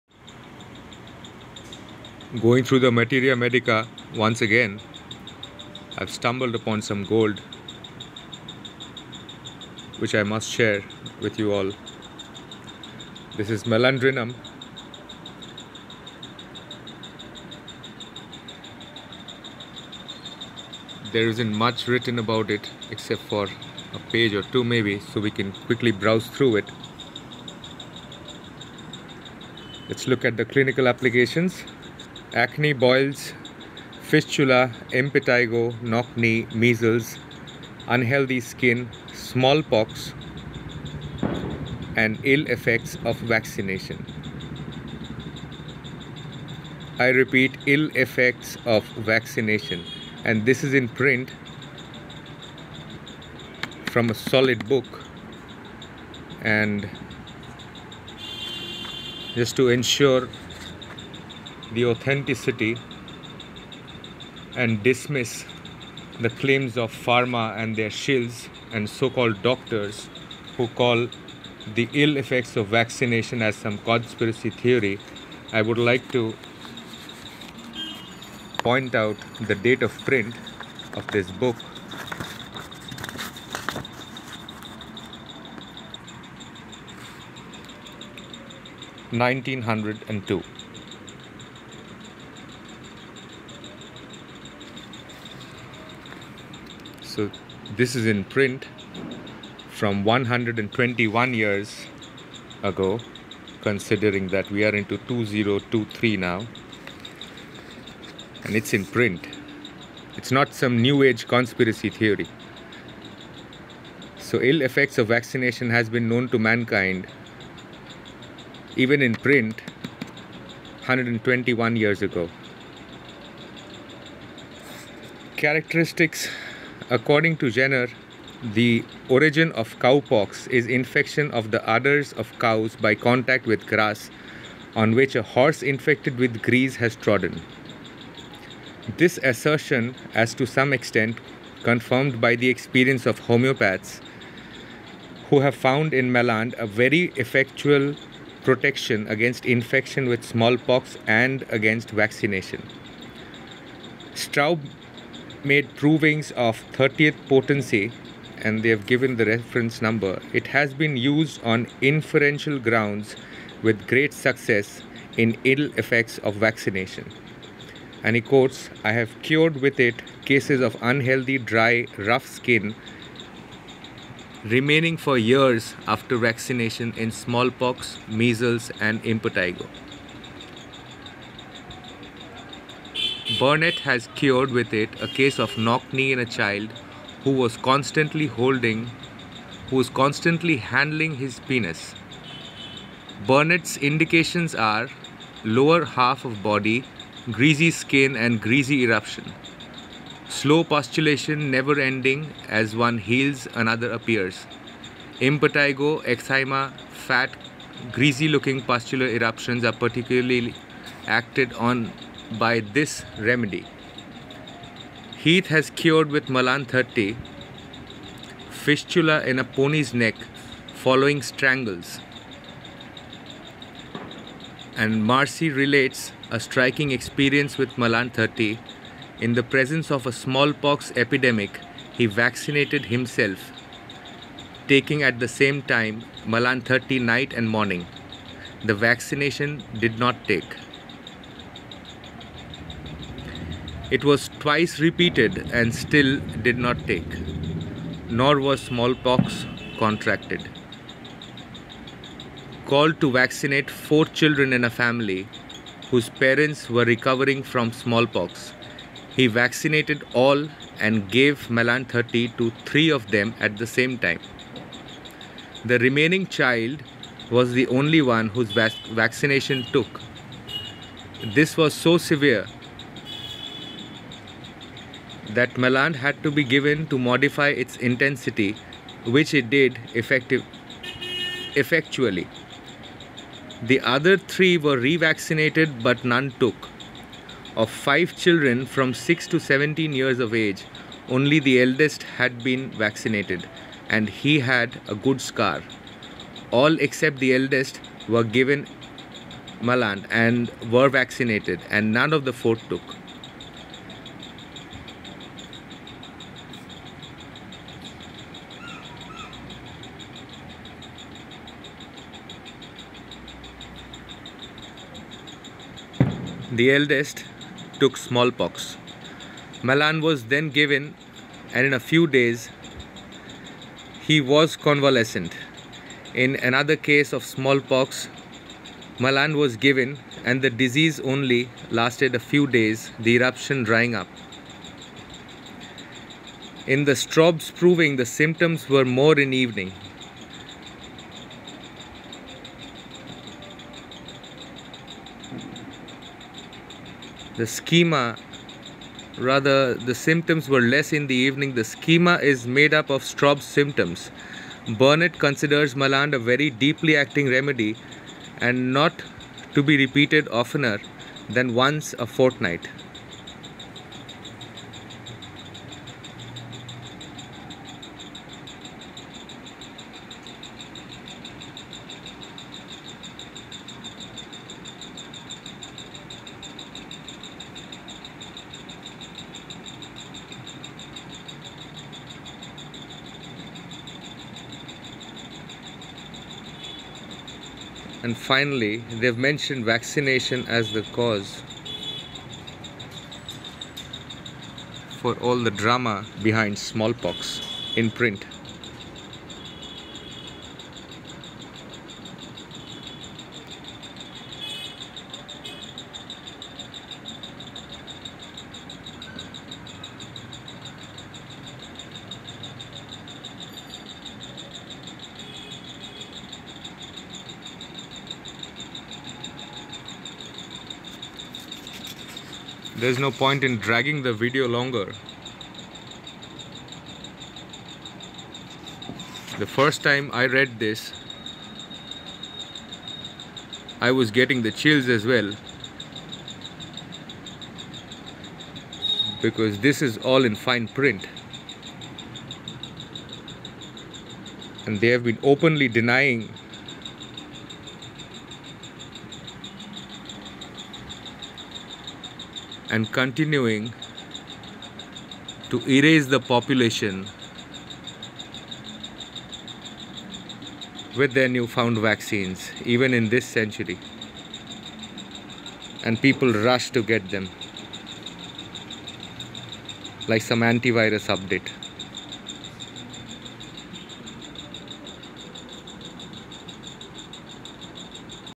Hard Copy Materia Medica 1902. Malandrinum.
CAUSATION: Vaccination. [As Published in the Hard Copy Book from 1902]
Following are the SYMPTOMS, as Published in the Book from 1902:
Head.-Dizziness.-Frontal and occipital headache.-Dulness.-Impetigo covering head from crown to neck and extending behind ears.-Thick, greenish crusts with pale, reddish scabs, itching lesser in evening.
Eyes.-Red stripes under eyes.
Ears.-Profuse, purulent, greenish-yellow discharge, mixed with blood.
Mouth.-Tongue: coated yellow, with red streak down middle (typhoid); cracked and ulcerating down middle; swollen.
Stomach.-Vomiting of bilious matter; nausea.
Abdomen.-Pains around umbilicus.
Stool.-Dark, cadaverous-smelling stool.-Yellowish, foul-smelling diarrhoea.
Male Sexual Organs.-(Child constantly handles his penis.)
Female Sexual Organs.-Vagina closed with impetiginous crusts, yellowish-greenish-brown in colour.
Back.-Pain along back, as if beaten.
Limbs.-Sore pains in limbs and joints.-Run-arounds on nails of hands and feet.
Upper Limbs.-Impetiginous crusts on extensor sides of forearms.
Lower Limbs.-Pains, esp. in l. tibia, with petechiae-like patches on anterior aspect of l. leg from knee to ankle.-Petechiae on both thighs,  l.-(Knock-knee.)
Skin.-Small-pox.-Measles; also as preventive.-Impetigo covering back of head, extending over back to buttock, and even into vagina; covering labiae.-Impetigo on extensors of forearms.-Boils.-Malignant pustule.-Bad effects of vaccination (dry, harsh skin).-Small, dusky red spots on legs, not disappearing on pressure.-(Typhoid fever.-Petechial typhus.)